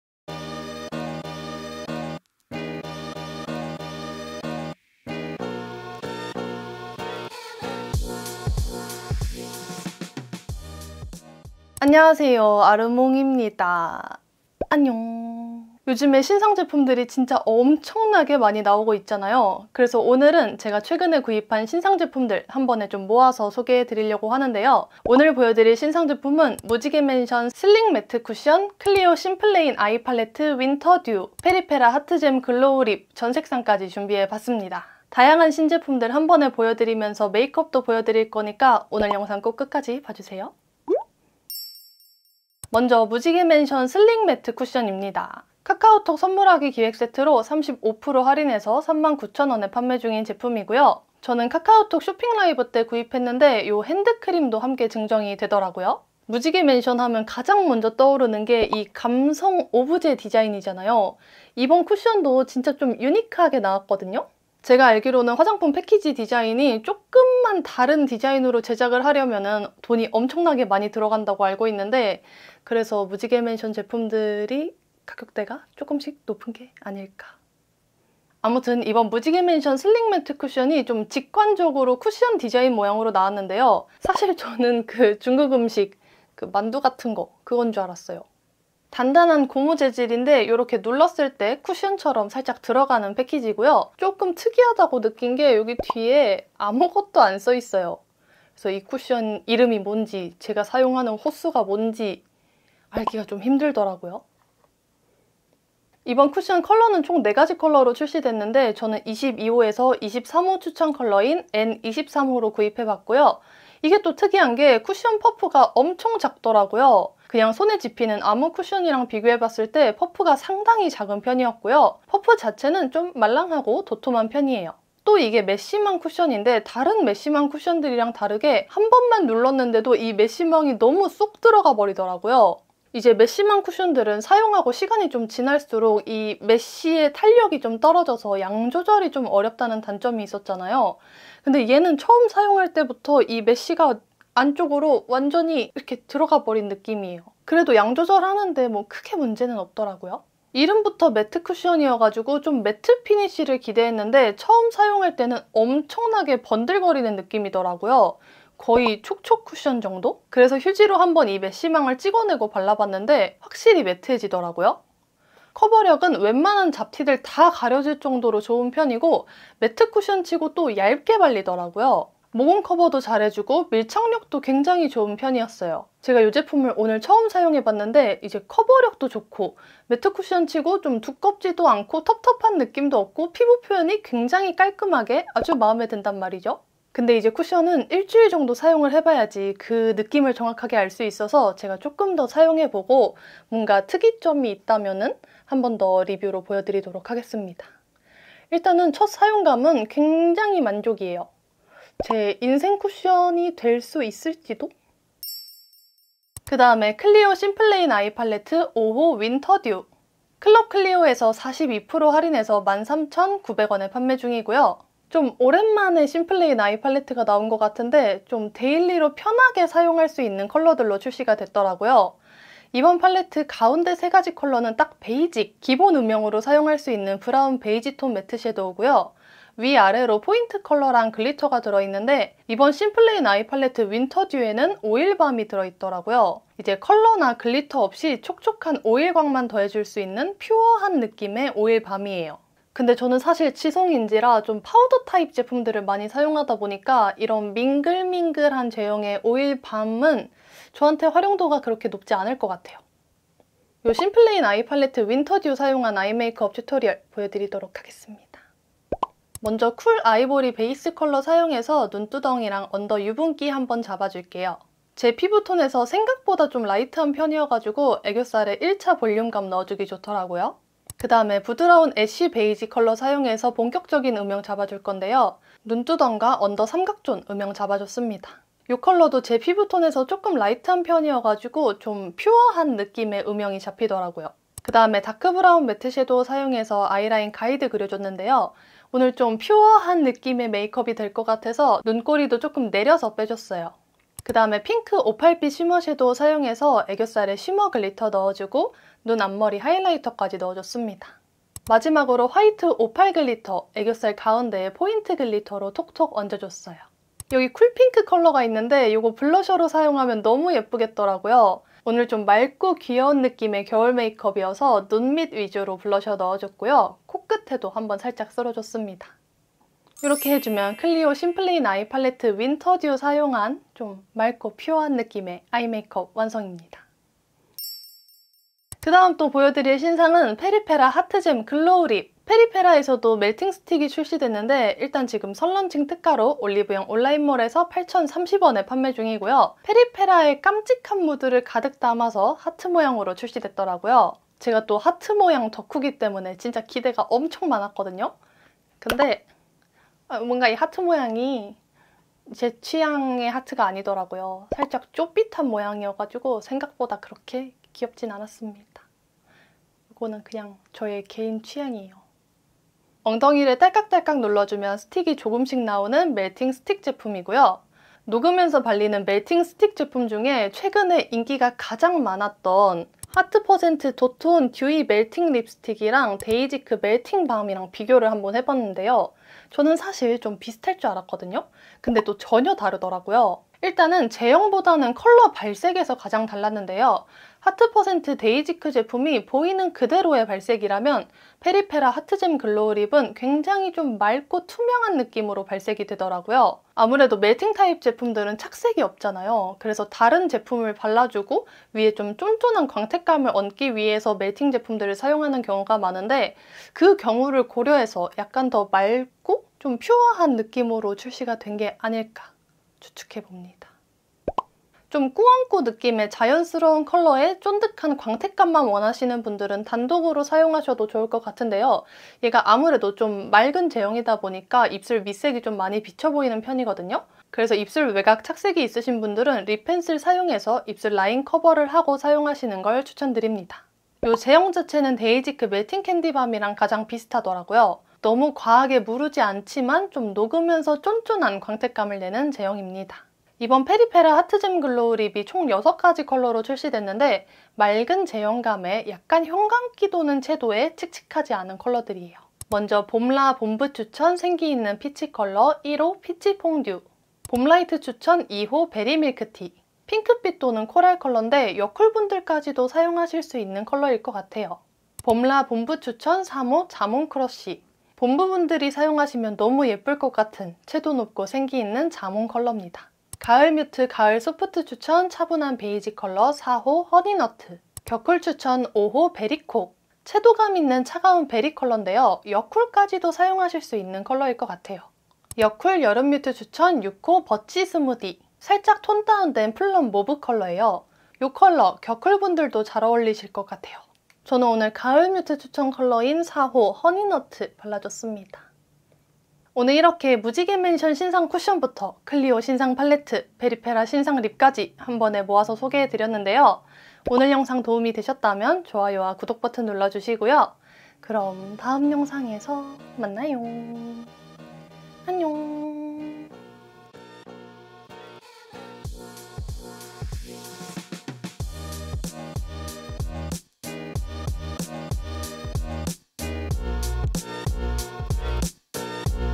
신상몽땅리뷰💸 멜팅립 제형 비교💄 무지개맨션 슬릭 매트 쿠션, 클리오 심플레인 아이팔레트 윈터듀, 페리페라 하트잼 글로우 립 | 아르몽
안녕하세요 아르몽입니동💖 아뉘~~~ 요즘 신상들 정말 무슨일이래요..? 통장 남아나질 않것어,,

00:00 영상시작
00:42 무지개맨션 슬릭 매트 쿠션
03:26 클리오 심플레인 아이 팔레트 05 윈터듀
05:28 페리페라 하트잼 글로우 립